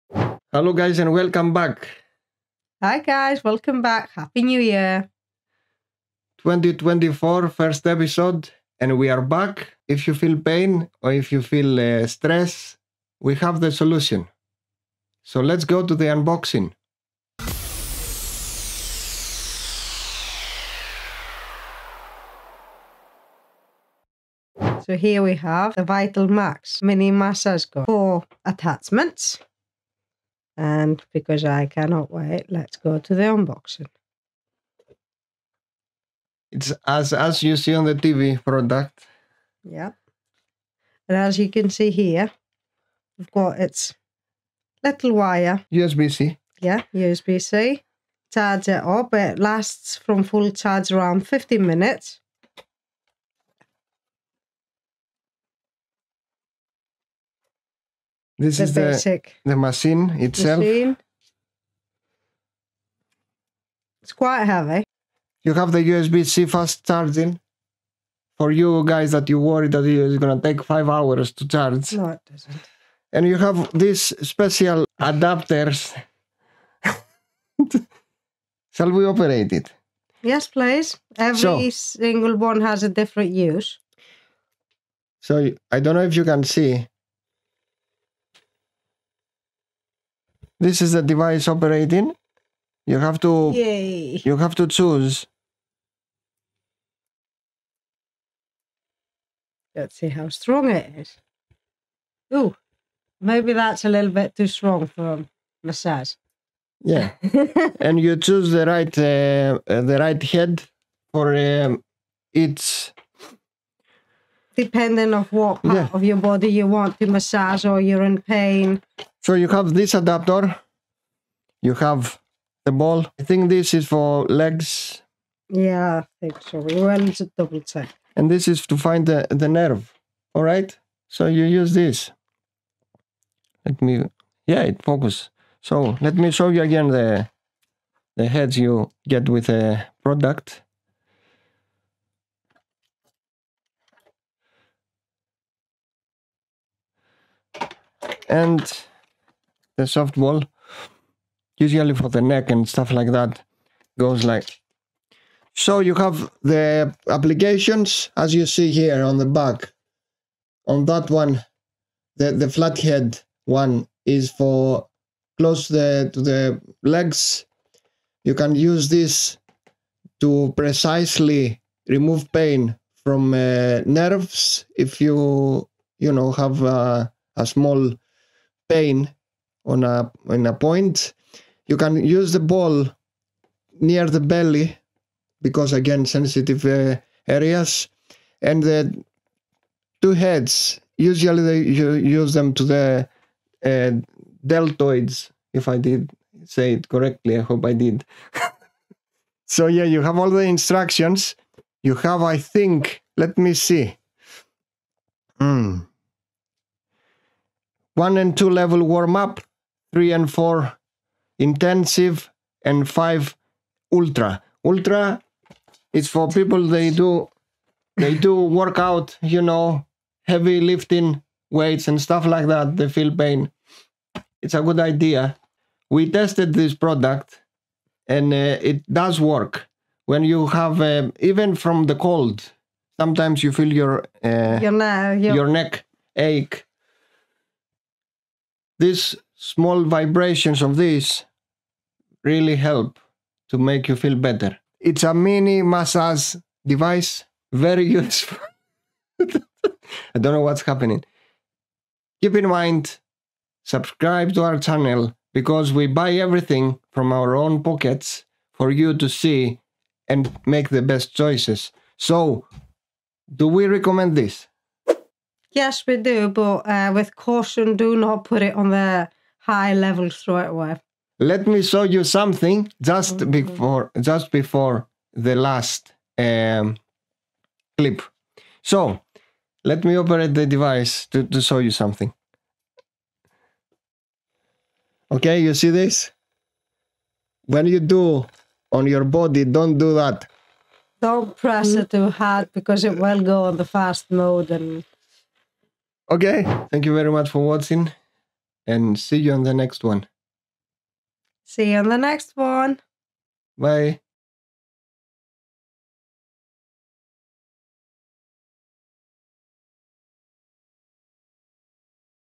Top Massage Gun Vitalmaxx | 3000 Strokes Per Minute Unboxing
Best massage gun handheld Physiotherapy Massager Gun - Professional Deep Tissue Physio Massaging Gun Relief of Muscle Tension

0:00 Welcome Zoe & Mr Dee
0:10 What can i do to stop the pain
0:24 This is a pain relief device for you
0:35 What's in the box
1:34 let's see the device working
2:20 what is included with this massage device
3:00 In what areas you can use it
3:29 what level shall be used
3:49 conclusion about this device
4:11 what's next ?